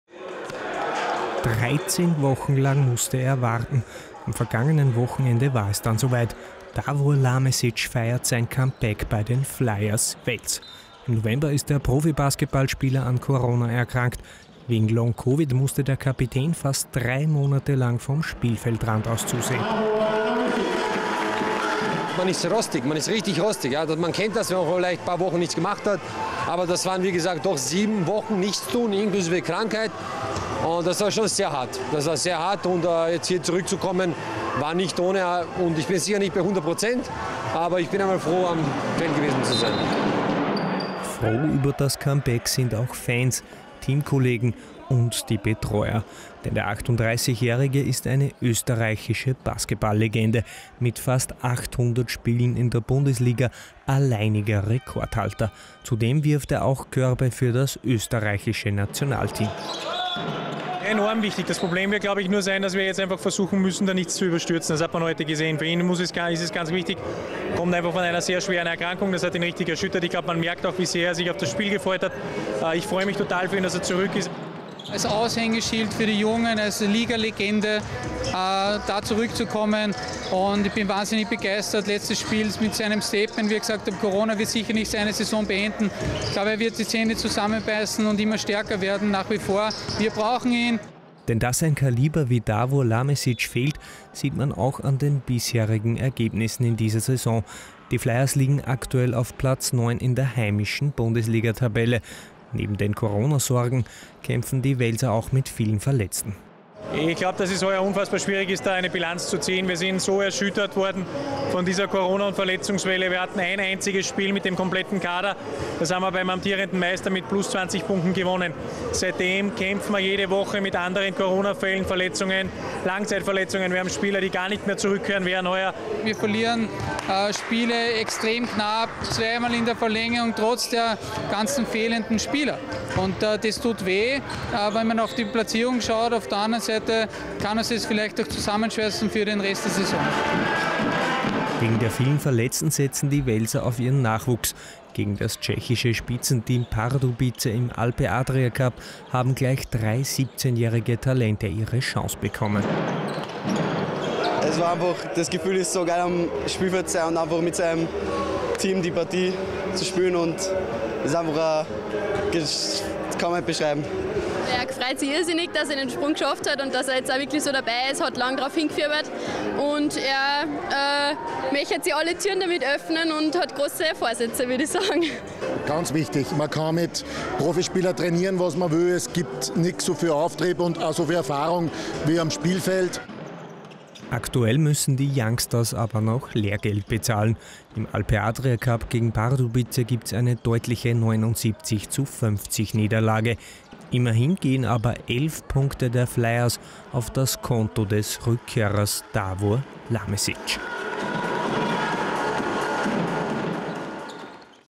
Absagen-Flut: Omikron hat Sport im Würgegriff
Der FC Bayern München, die Black Wings Linz oder auch die Basketballer von den Flyers Wels. Egal welche Sportart. Aktuell kämpfen alle mit Corona-Clustern, Absagen oder auch Long-Covid Ausfällen.